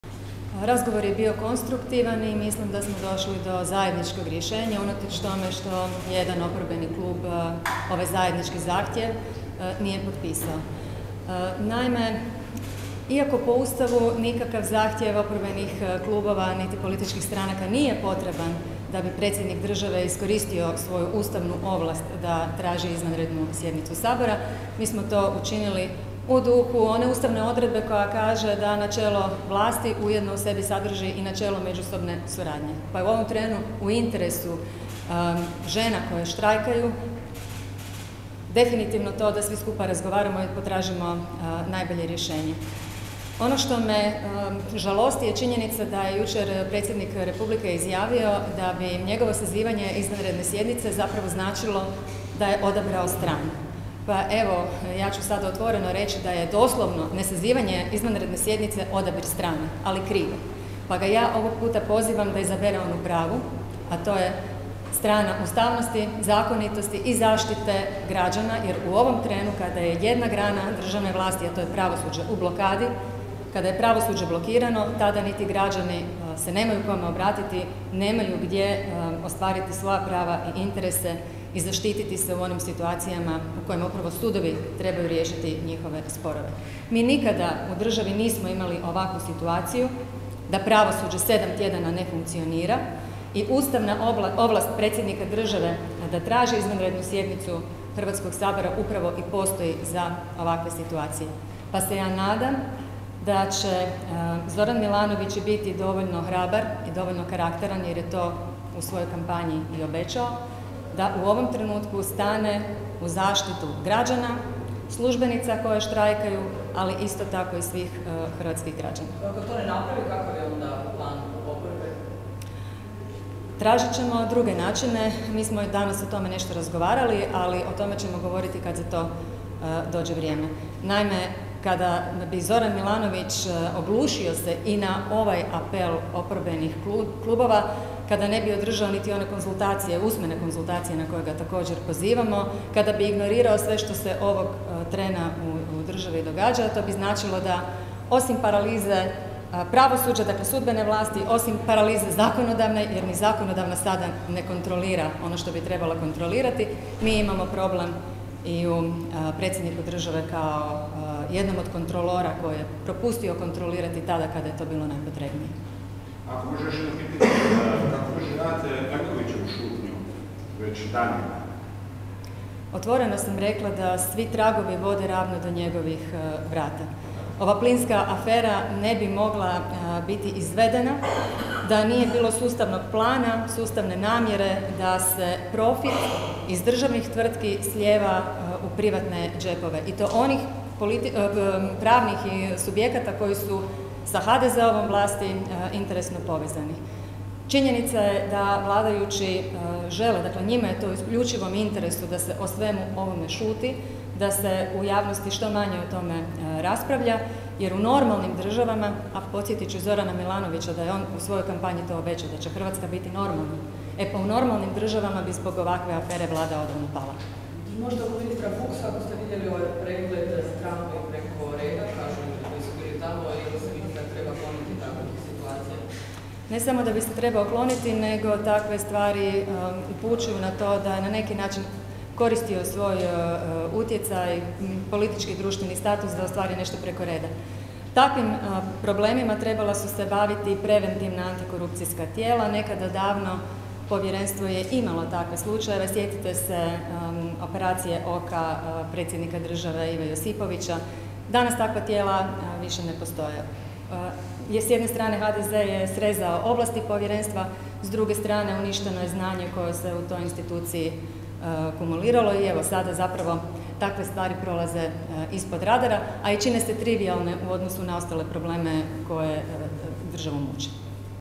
Orešković: "Pozivam predsjednika Milanovića da izabere pravu stranu"
U Saboru je održan još jedan u nizu sastanaka predstavnika oporbenih stranaka. Nakon prošlotjednog završetka ljetne plenarne sjednice Sabora čelnici stranaka pokušavaju pokazati da nisu otišli na godišnje odmore i da će raditi cijelo ljeto. Nakon sastanka održana je konferencija za medije na kojoj su glavne teme bile političke aktualnosti, plinska afera i štrajk službenika na sudovima. Izjava Dalije Orešković.